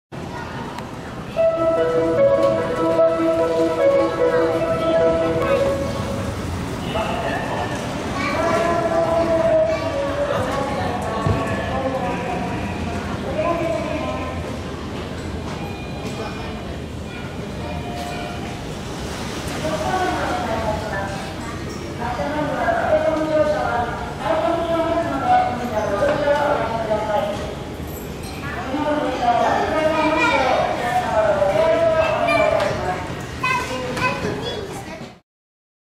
三ノ宮駅（ポートライナー）エスカレーター ホーム階から改札階 三菱製 Kobe Airport station Escalator Mitsubishi
神戸新交通ポートアイランド線、三ノ宮駅のエスカレーター

【amazonリンク】